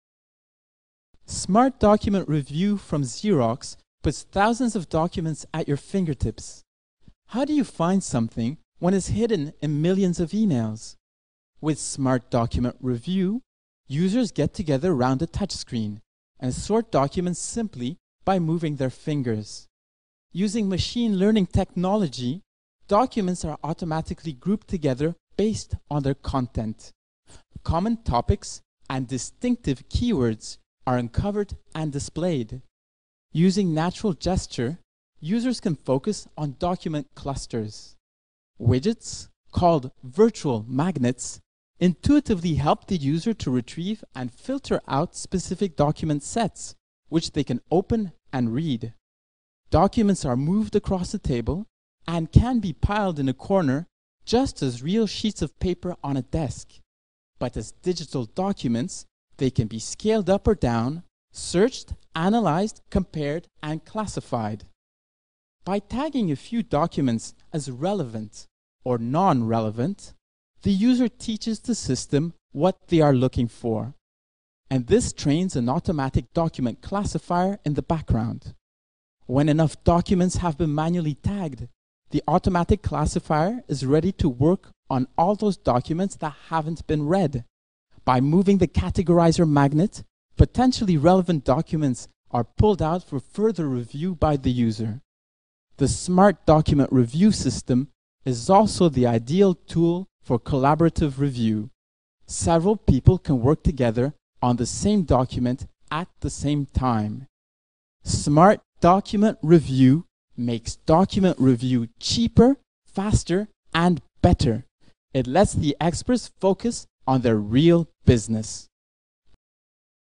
Сенсорный стол от Xerox
Система Smart Document Review представляет собой интерактивный столик с большим горизонтально расположенным сенсорным дисплеем. Сортировкой документов могут заниматься несколько пользователей одновременно, для этого в системе предусмотрен целый набор мощных инструментов.
К примеру, можно создавать так называемые «магниты», которые позволяют выбирать из общей кипы документов только те, что содержат заданные ключевые слова.